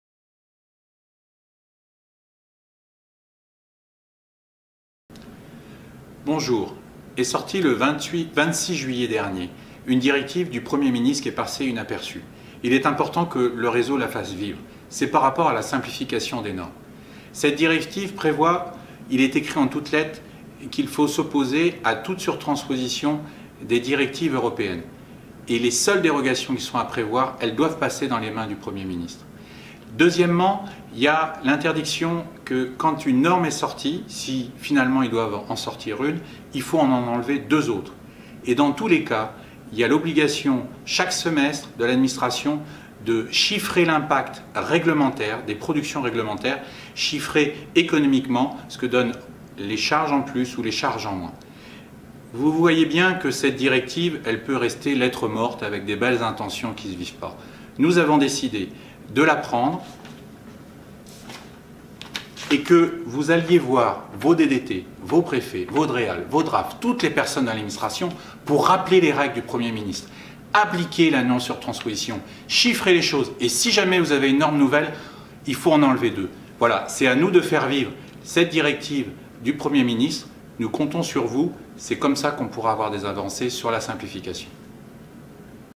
Simplification : mobilisez-vous ! Le message d'Eric Thirouin
Dans une directive de juillet 2017, le Premier Ministre prend des garanties sur la simplification des normes ! Mobilisons nous pour que cette directive soit appliquée